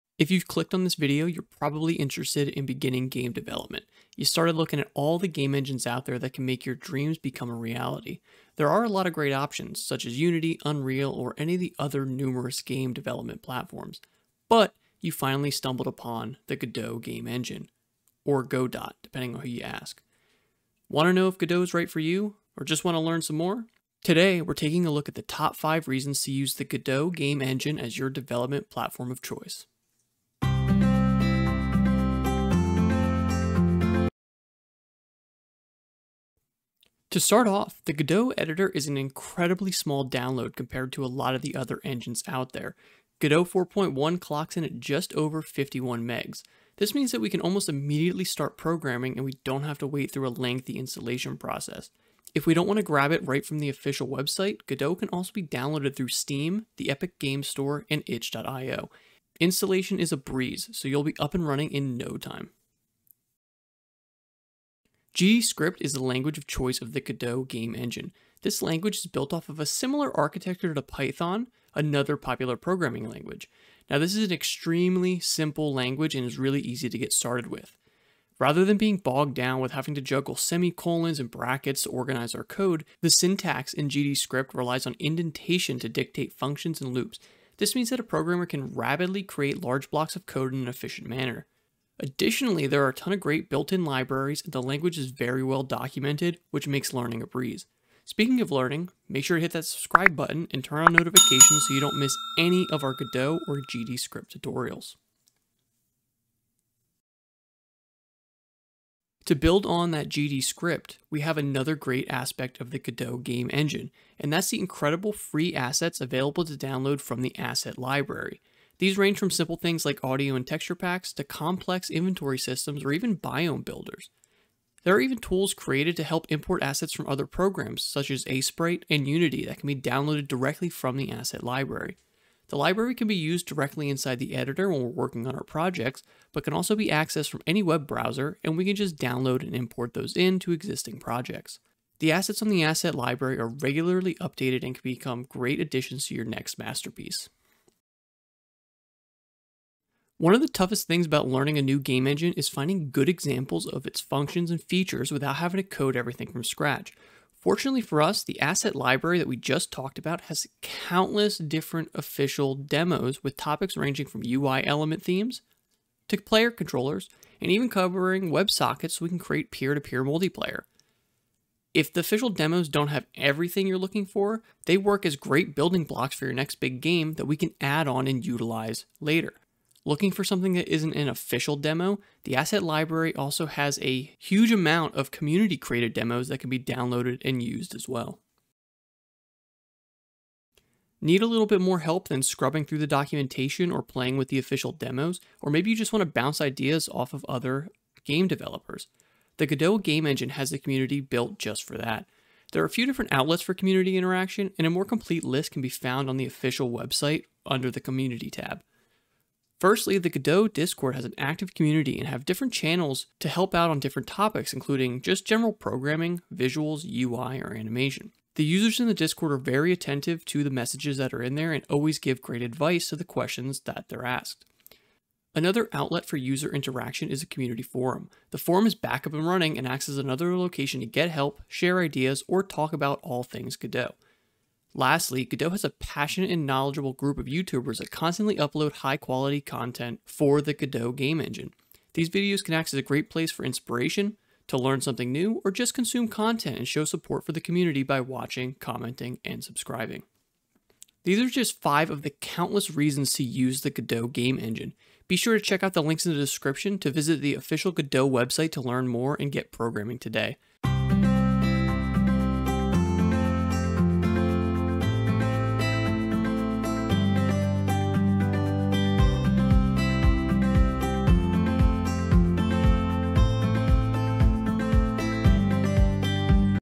Top 5 Reasons to Use Godot to Make Games
Watch this to learn the Top 5 reasons why you should consider using the Godot Game Engine for your game development projects.

Are you searching for the perfect game engine to bring your creative visions to life? Look no further than Godot! In this video, we will delve into the key features and advantages that make Godot a standout choice for game developers.

00:00 Intro
00:34 Small Download
01:02 GDScript
01:46 Asset Library
02:27 Official Demos
03:07 Active Community
04:10 Outro